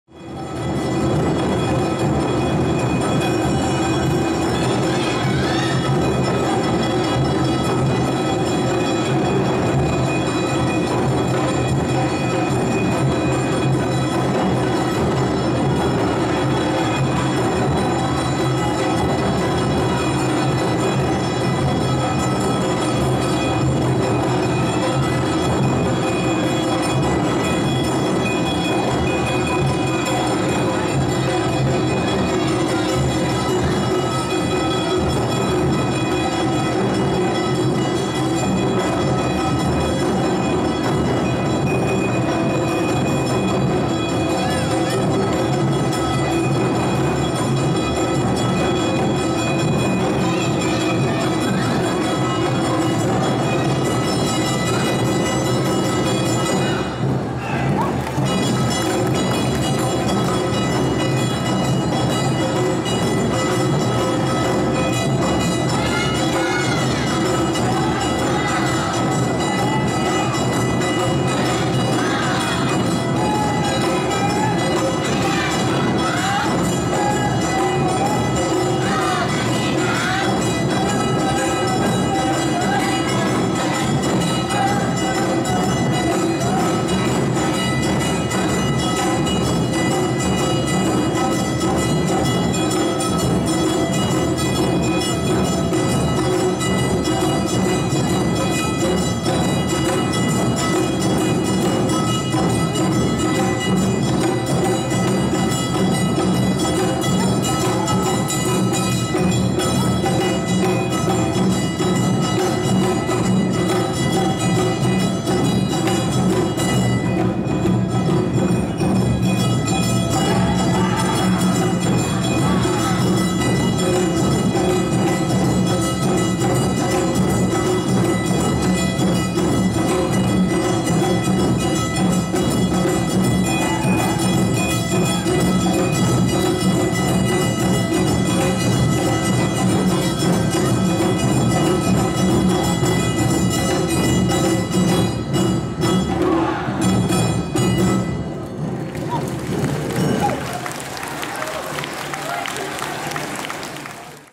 阿波踊り「下北沢 やっとこ連」第32回かわさき阿波おどり（2017.10.7）
第32回かわさき阿波おどり　　雨天決行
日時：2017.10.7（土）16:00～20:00
会場：JR川崎駅東口（銀座街・銀柳街・ルフロン・ラ チッタデッラ）
撮影メモ：FDR-AX55　4K　銀柳街で撮影

more...  阿波おどり動画日本一！の動画ブログ はこちら。